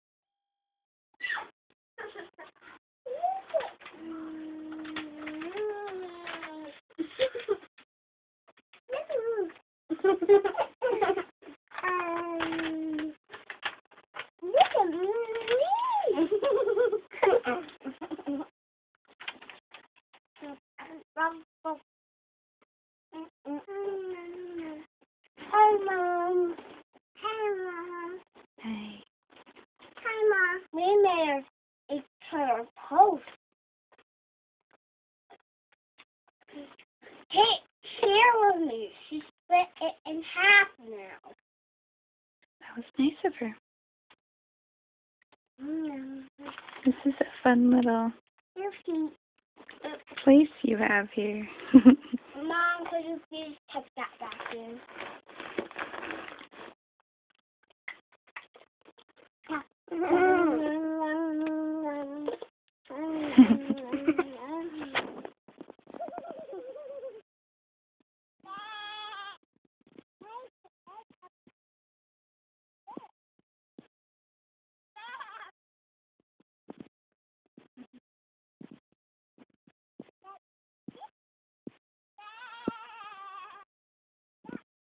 Bunk bed fort for Lily and Kate
The girls create a hideout where they cab eat toast. This was one of those moments for me where I had to take a deep breath and look at the bigger picture and not make a big deal out of something small. I believe the reason I went to check on them was because the lamp was missing from the front room. And no, I don't usually allow toast in bed.